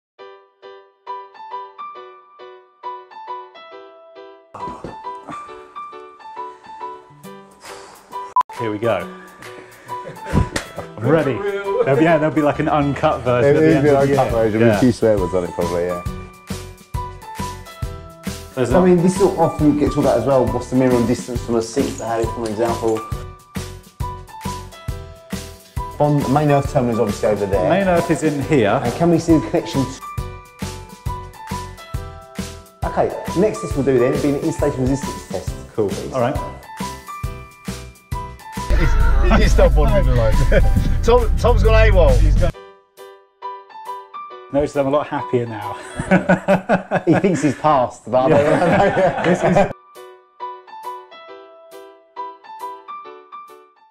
My NICEIC assessment - sneak peek | Thomas Nagy
Ever wondered what actually happens on an NICEIC yearly assessment? A sneak peak into my annual assessment. Full version out on 23rd July at 8pm!

Thomas Nagy